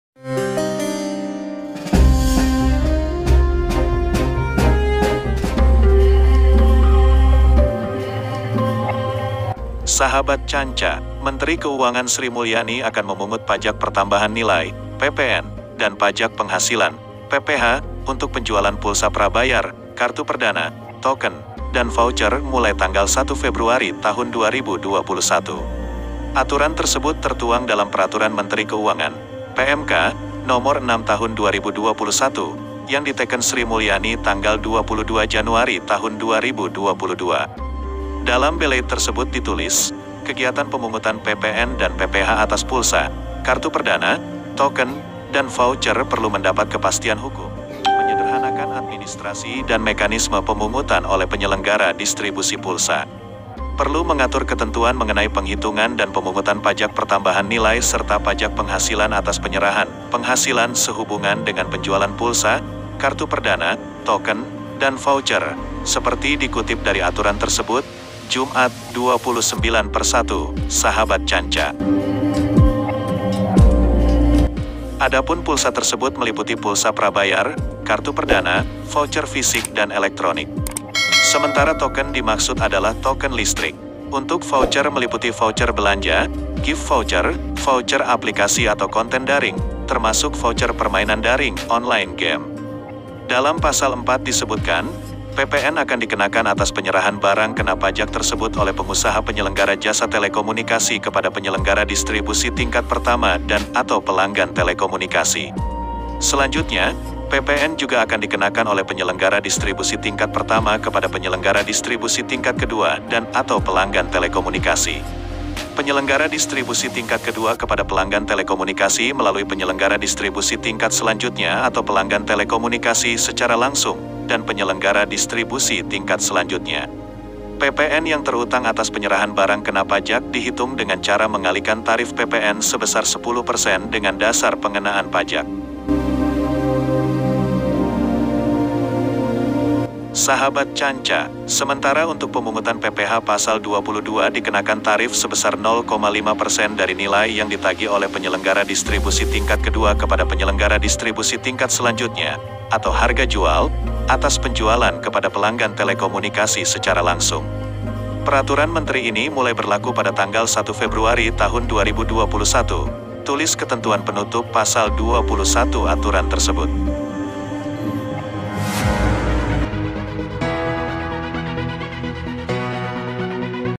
Sri Mulyani Pungut Pajak Jualan Pulsa Hingga Voucher Belanja 1 Februari
"Canca" Channel merupakan dibangun untuk mengungkap fakta dan kisah-kisah unik yang mungkin jarang teman-teman ketahui

sri mulyani#canca